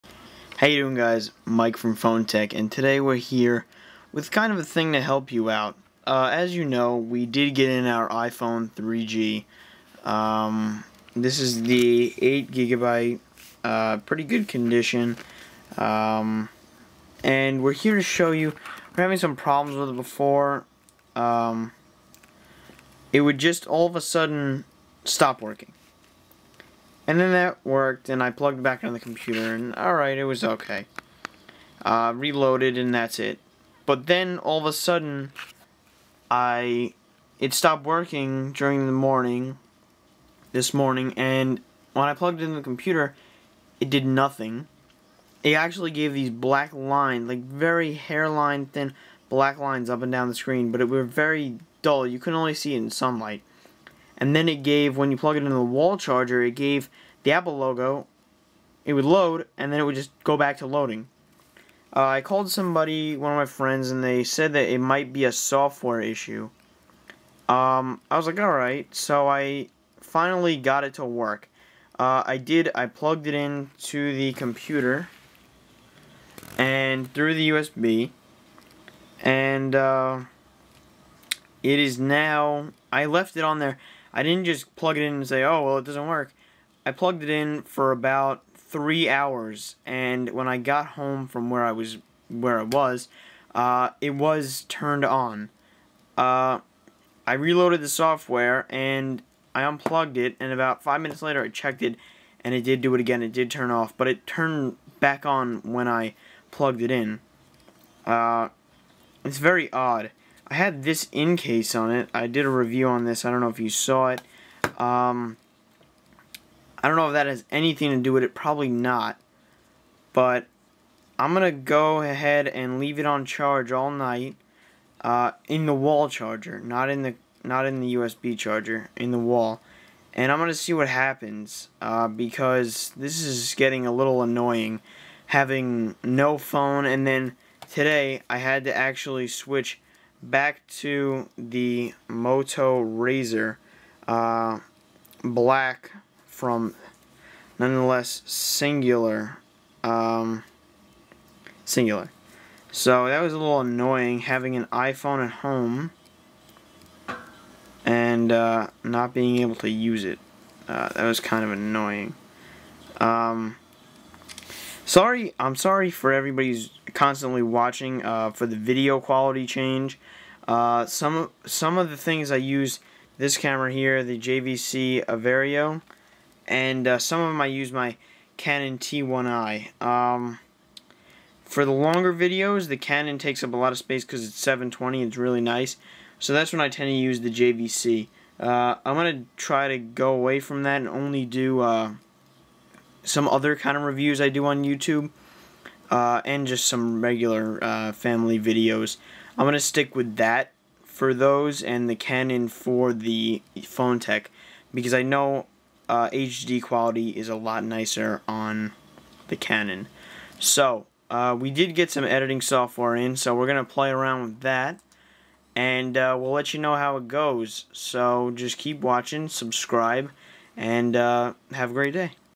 iPhone 3G Black Lines in screen Quick Fix
All i did was leave it plugged into the usb for 3 hours and it finally turned on and i restored it. It was inconvenient and i will put a wall post on you tube if it happens again.

Thanks